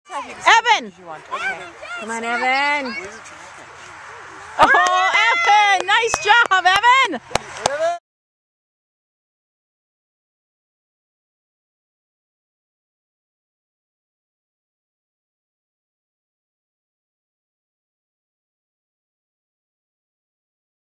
Evan Scores
Evan scores his first U10 goal from a PK!